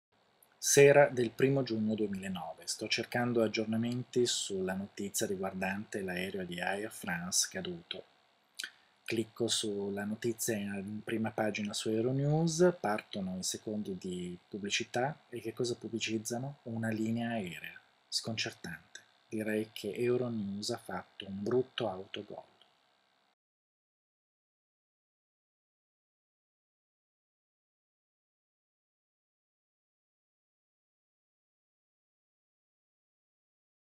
Euronews il disastro di Air France e lo spot
Euronews manda in onda uno spot di una compagnia aerea prima di dare la notizia del disastro aereo di Air France.